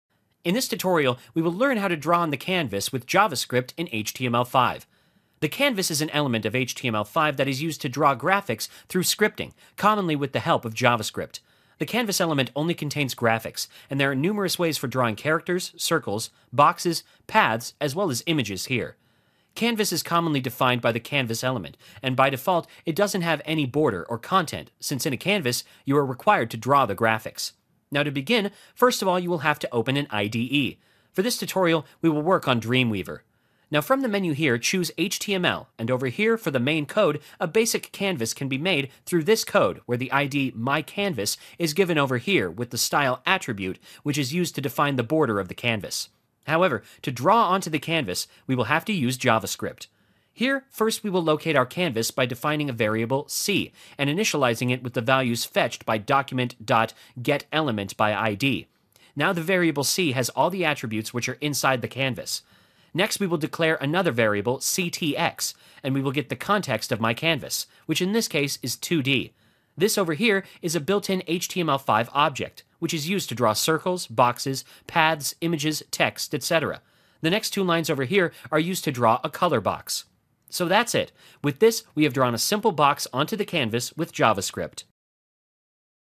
How to Draw onto the Canvas with JavaScript in HTML5
Follow this easy step by step tutorial to learn how to draw canvas with javascript in html5 using getcontext object.

HTML5 is the latest version of html, which is still under development. This tutorial is about how we can draw onto the canvas in JavaScript and html5. In this tutorial we will learn about html5 canvas and java script.

Step #1 - Open IDE

Now, to begin, first we will have to open an IDE, let's say Dreamweaver. Now, from its menu, choose HTML from Create New.

Step # 2 -- Basic Code

Now, for the main code, a basic canvas can be made through this code, where the style attribute is used to define the border of the canvas.

Step # 3 -- Using JavaScript

To draw onto the canvas, we will use JavaScript.

Here, first we will locate our canvas, next we call getContext("2d") object, which is a built-in HTML5 object used to draw circles, boxes, paths, images, text, etc.

Next two lines are used to draw a color box.

This is how we can work with Html5, canvas and java script collectively to draw objects.